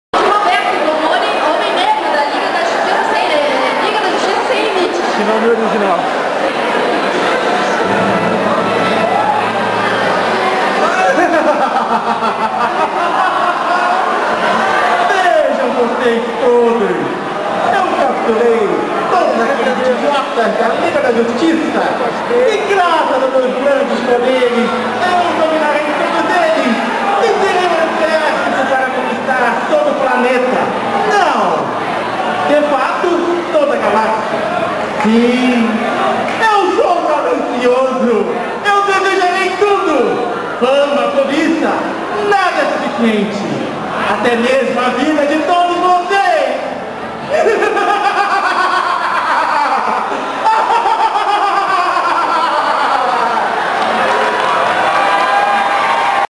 AnimePlace Abril2007-Cosplay Individual-CR-Homem-negro-LJSM
Apresentação no AnimePlace de Homem Negro, vilão da Liga da Justiça sem Limites.